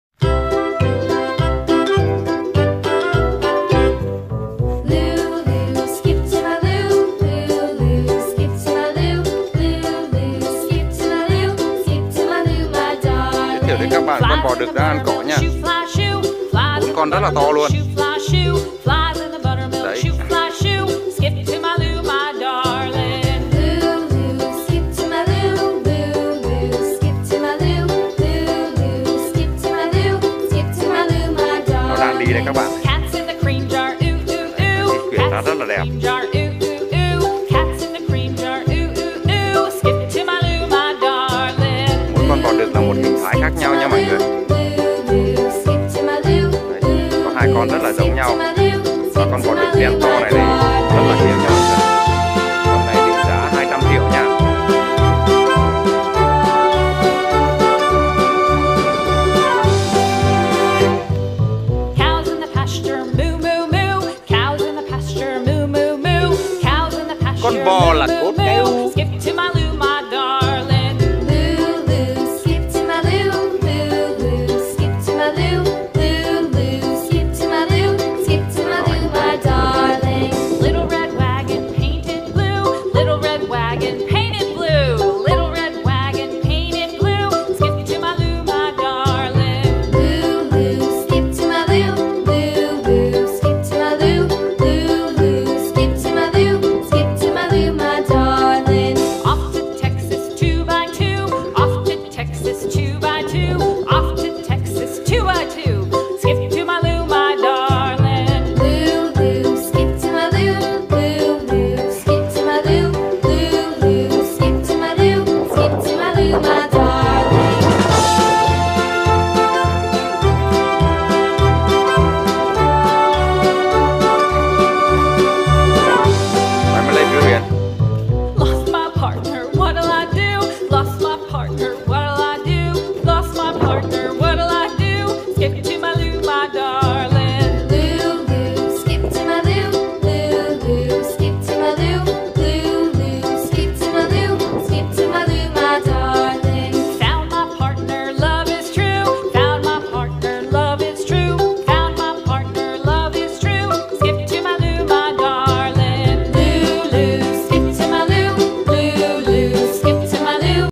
4 con bò đực siêu đẹp , cùng đàn bò đi học ở trường mẫu giáo rất dễ thương
Đàn bò vàng cùng 4 con bò đực siêu to..